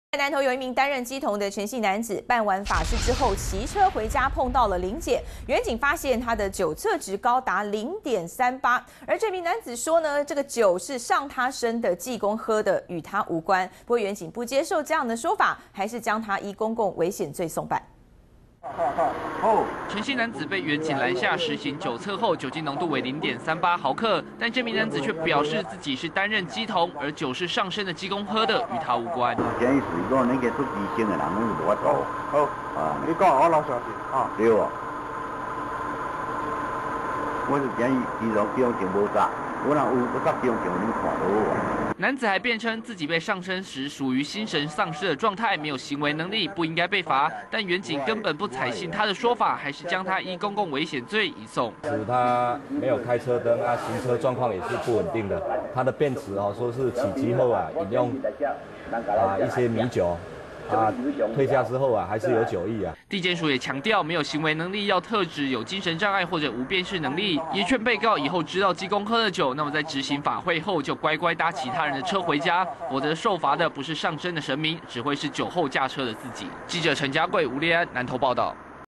[中視新聞] "都是神明喝的!" 乩童酒駕照抓送辦 20140414
南投一名擔任乩童的陳姓男子，在辦完法事後騎車回家遇到臨檢，員警發現他酒測值高達0.38，沒想到這名男子竟然說酒是上他身的濟公喝的，與他無關，不能判他罪，但員警並不採信他的說法，還是將他依公共危險罪移送